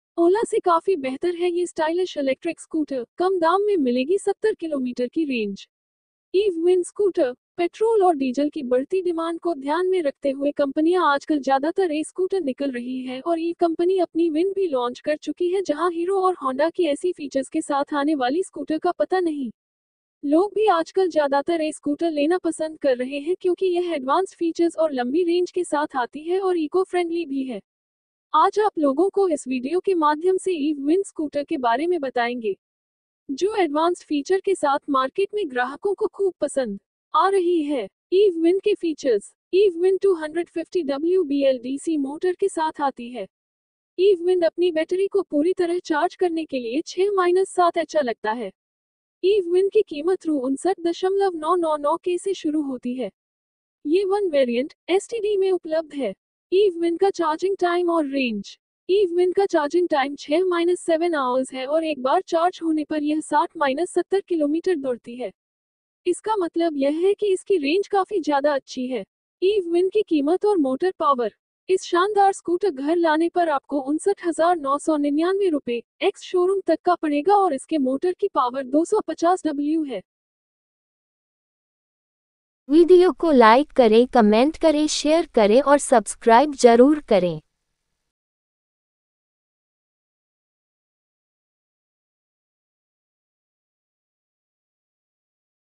EeVe Wind Price, Range, Battery Charging Time, Top Speed | eeve wind launch New Electric scooter
– Key Specs & Features of Wind

- Range : 60-70 km/charge
- Battery Capacity : 1.62 Kwh
- Kerb Weight : 94 kg
- Top Speed : 25 km/Hr
- Motor Power : 250 W
- Motor : BLDC

About EeVe Wind
The EeVe Wind is an electric scooter, available in 1 variant, at a starting price of Rs 59,999. The battery capacity of the Eeve Wind STD is 1.62 Kwh. The weight of the Wind is 94 kg.The Wind comes with a Disc brake up front and Drum brake at the rear. You can buy the Wind in 3 colours - Black, Red, White.

EeVe Wind Price
The price of EeVe Wind in India starts at Rs. 59,999 EeVe Wind is offered in 1 variant - Eeve Wind STD which comes at a price tag of Rs. 59,999.